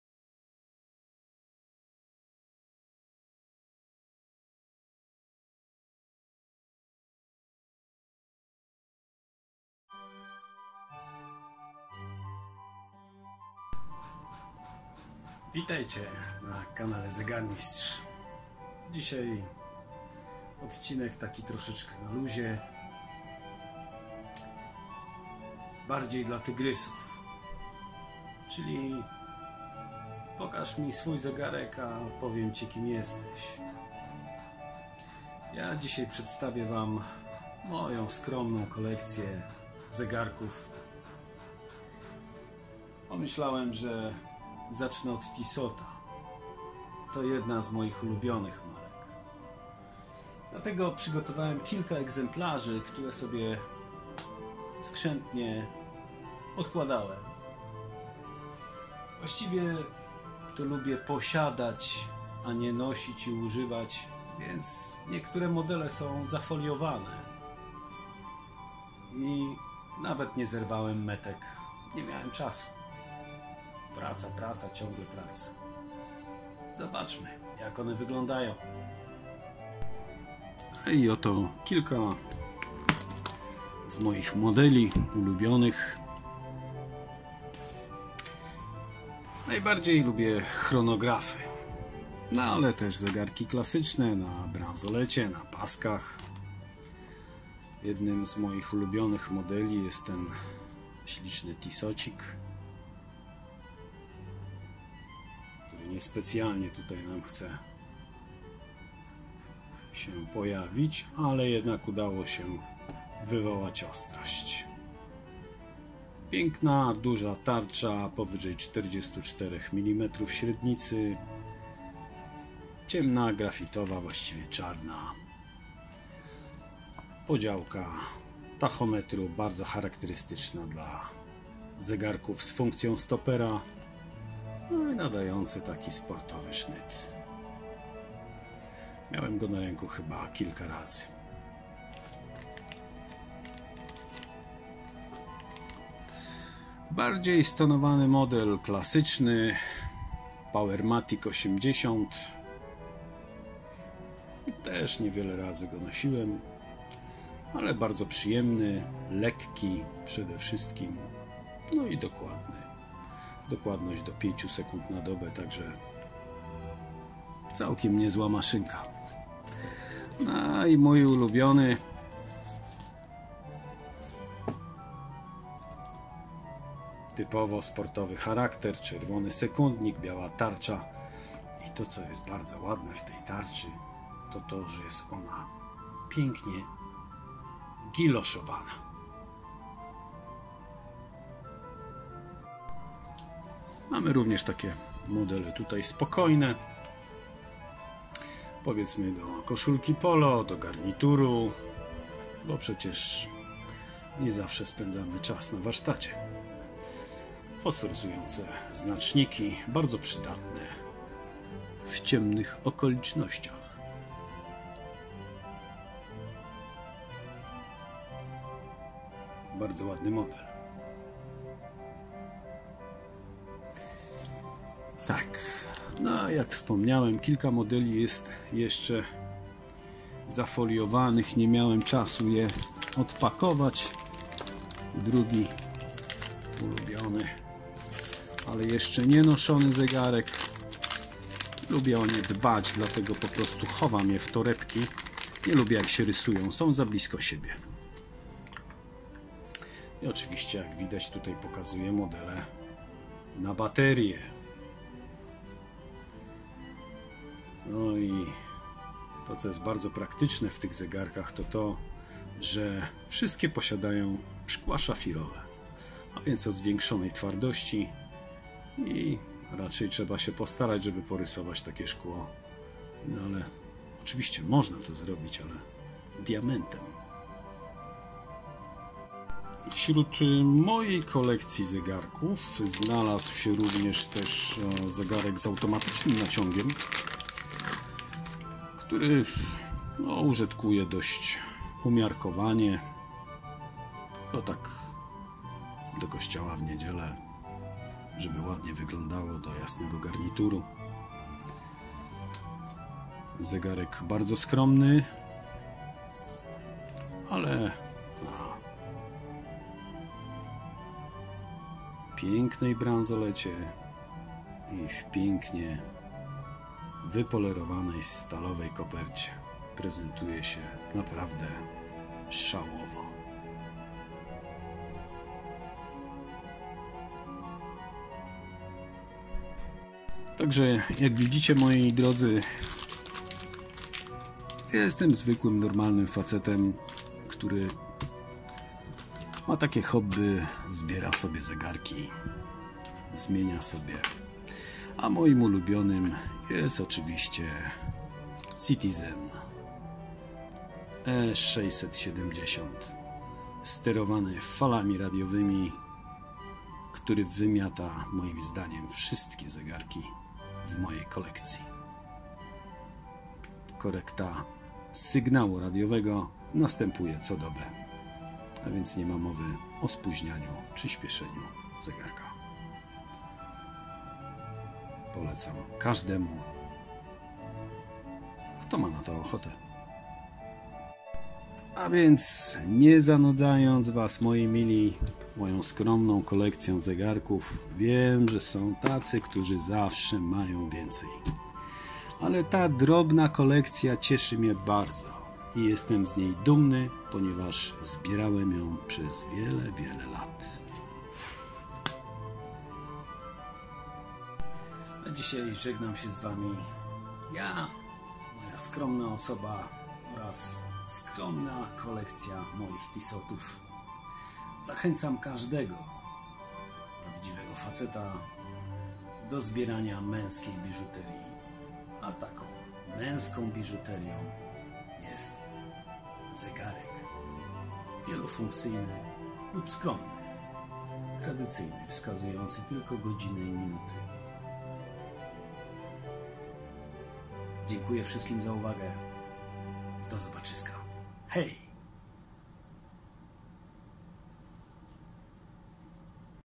Moja skromna kolekcja Tissot 😀Meine bescheidene Sammlung 🙂My Modest Collection
Moja skromna kolekcja zegarków Tissot nikogo nie powali na kolana, ale jestem z niej dumny :)